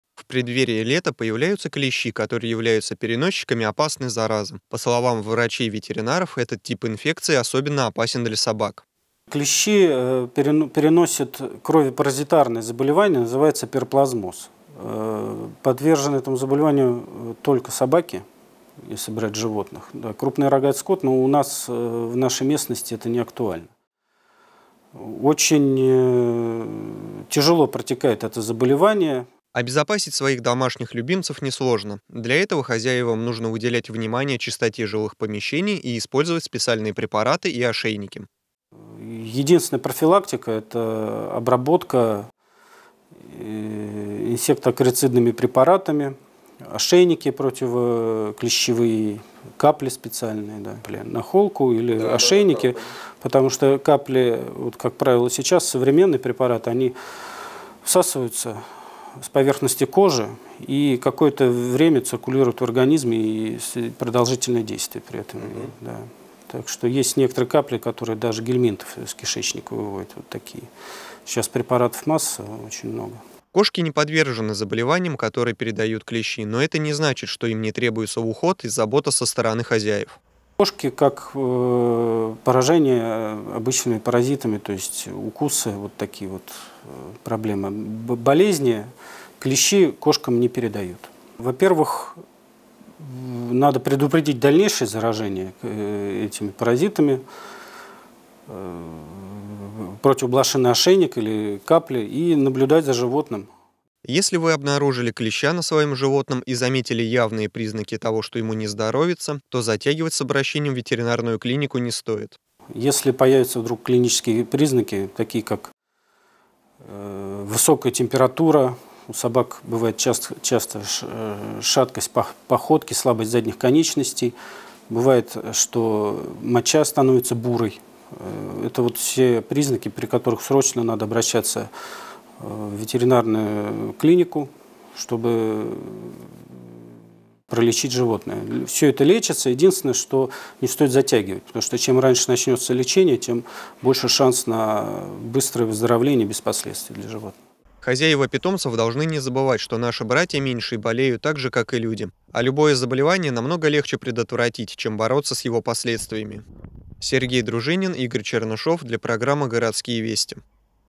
С наступлением весны нашим братьям меньшим требуется дополнительный уход и забота.
С наступлением весны нашим братьям меньшим требуется дополнительный уход и забота. Именно в эту пору хозяевам необходимо подумать о здоровье своих питомцев. Сотрудники Раменской ветеринарной станции дали свои пояснения и рекомендации о том, как обезопасить своих питомцев от  клещей.